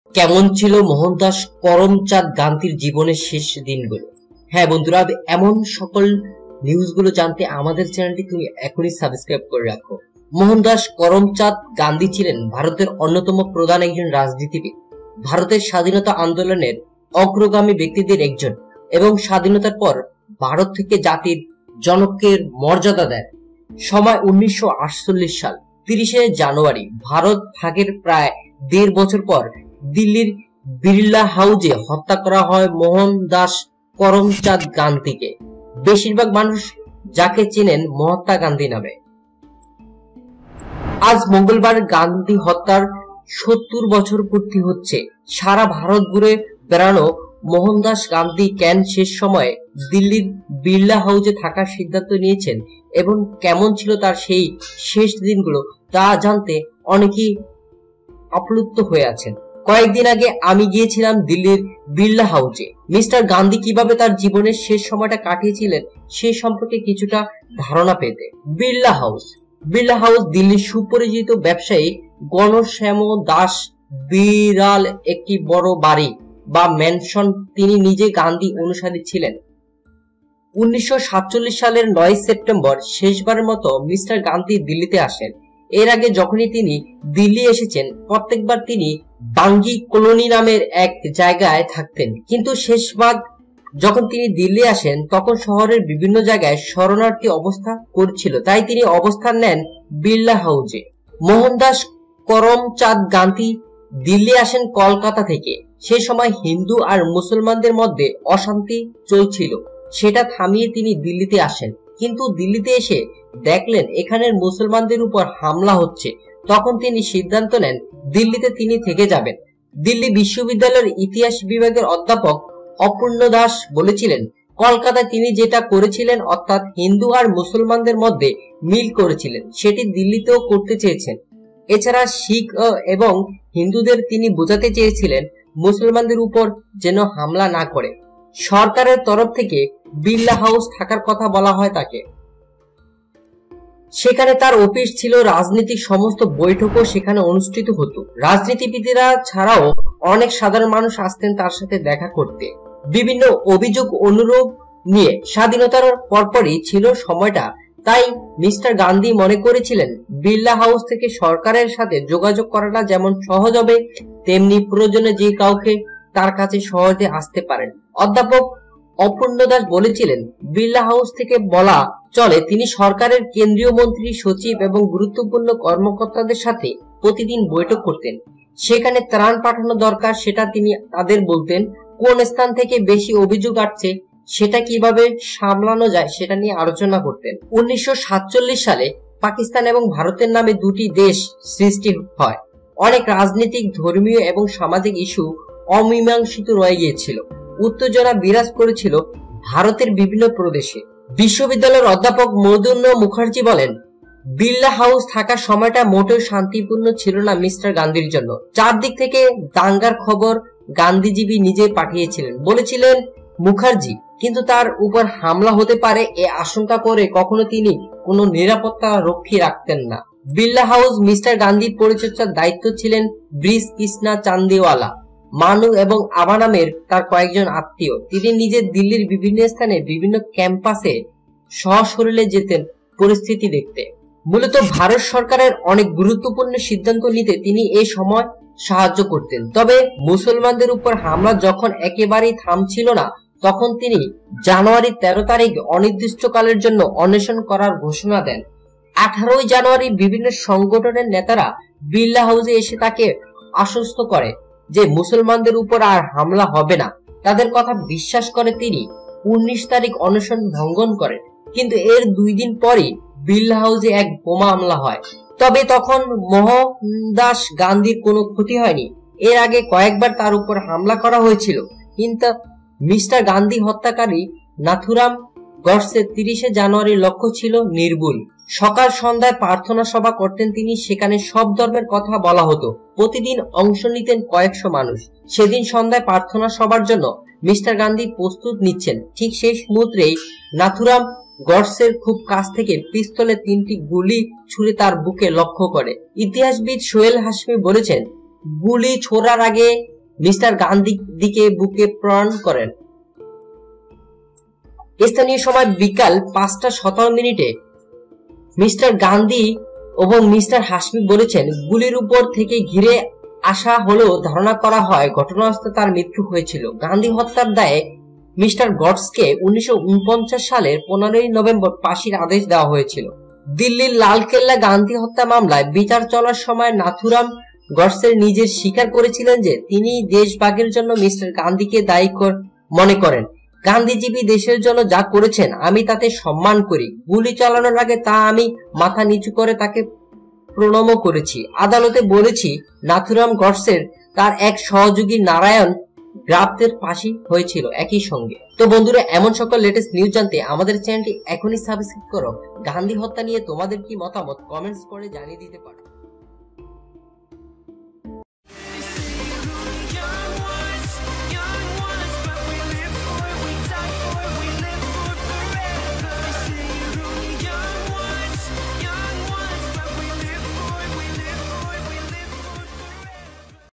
দেখুন কেমন ছিল মহাত্মা গান্ধী জীবনের শেষ দিনগুলো !
দেখুন কেমন ছিল মহাত্মা গান্ধী জীবনের শেষ দিনগুলো ! মোহনদাস করমচাঁদ গান্ধী ছিলেন ভারতের অন্যতম প্রধান একজন রাজনীতিবিদ, ভারতের স্বাধীনতা আন্দোলনের অগ্রগামী ব্যক্তিদের একজন, এবং স্বাধীনতার পর ভারত তাঁকে জাতির জনকের মর্যাদা দেয়।

সময় ১৯৪৮ সাল। ৩০শে জানুয়ারি। ভারত ভাগের প্রায় দেড় বছর পরেই দিল্লির বিরলা হাউজে হত্যা করা হয় মোহনদাস করমচাঁদ গান্ধীকে, বেশীরভাগ মানুষ যাকে চেনেন মহাত্মা গান্ধী নামে।

আজ মঙ্গলবার গান্ধী হত্যার ৭০ বছর পূর্তি হচ্ছে।

সারা ভারত ঘুরে বেড়ানো মোহনদাস গান্ধী কেন শেষ সময়ে দিল্লির বিরলা হাউসে থাকার সিদ্ধান্ত নিয়েছিলেন এবং কেমন ছিল তাঁর সেই শেষ দিনগুলো তা অনেকের কাছেই অজানা।

কয়েকদিন আগে আমি গিয়েছিলাম দিল্লির বিরলা হাউজে, মি. গান্ধী কীভাবে তাঁর জীবনের শেষ সময়টা কাটিয়েছিলেন সে সম্পর্কে কিছুটা ধারণা পেতে।

Next Video:
► অ্যালকোহল বাড়াতে পারে স্মৃতিশক্তি ! Alcohol Could Raise Memories ! Bengali TV